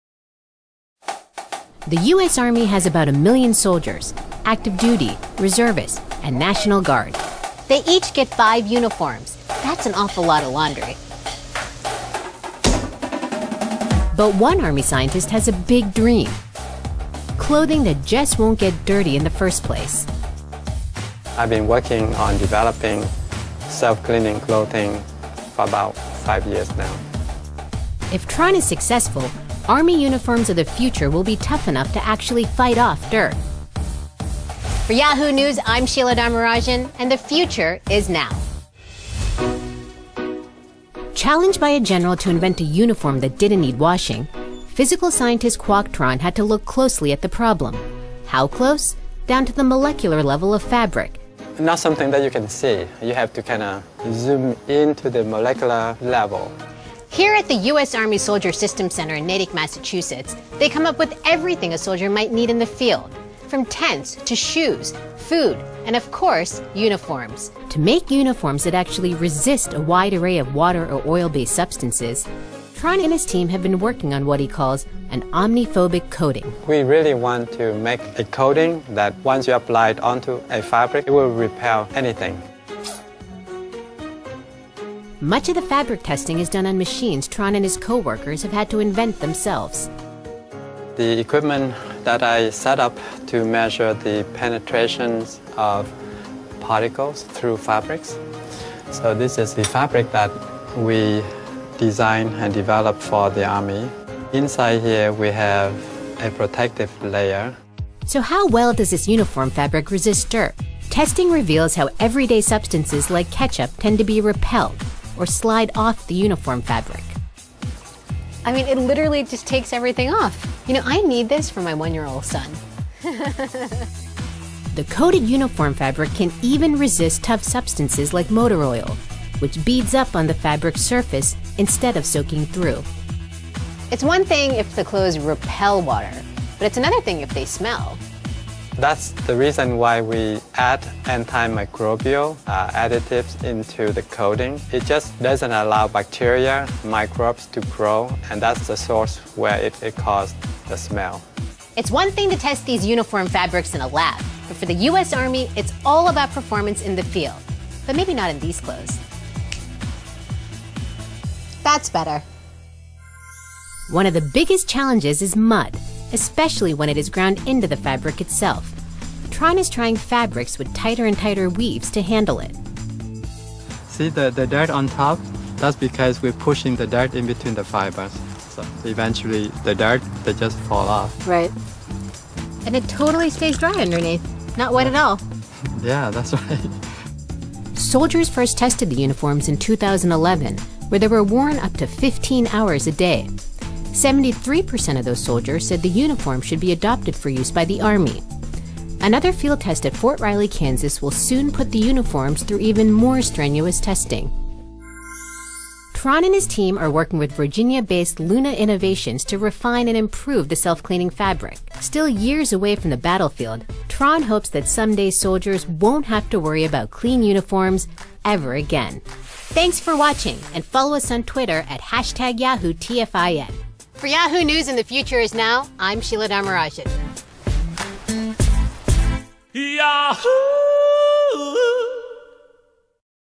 NOVEDAD - Uniformes sin manchas repelentes a la suciedad
Tratamiento para téxtil omnifóbico  de alta duración . Repelente de agua , suciedad y todo tipo de líquidos y aceites. Repelente superhidrófugo para  facilitar la limpieza del día a día. Este tratamiento ya se usa en los uniformes del ejército de los Estados Unidos.